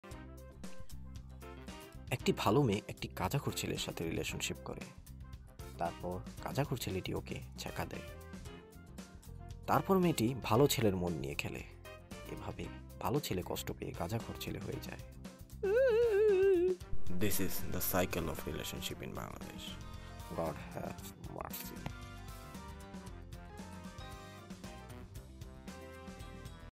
Bengali Philosophy - The Cycle of Relationships in Bangladesh
Script and Animation : Reyan Onto
Voice Over : Ariyan Anom